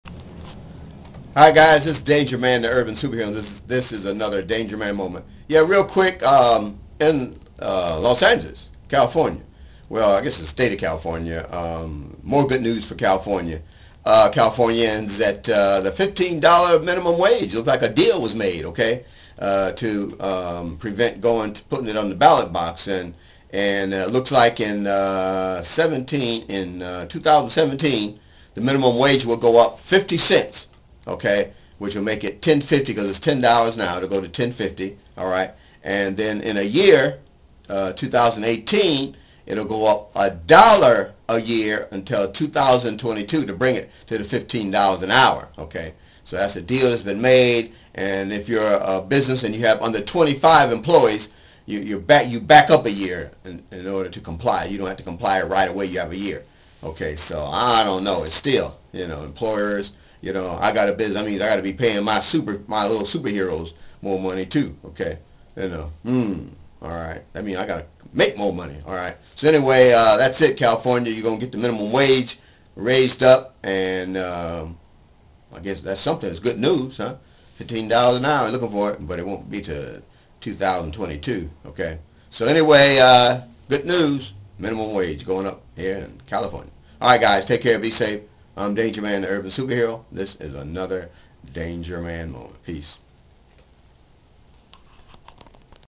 $15.00 an Hour Comes to CA Minimum Wage Deal Reached!
Breaking News Minimum Wage increase $15.00 increase in 5 years will avoid the ballot box. Yeah for CALLY!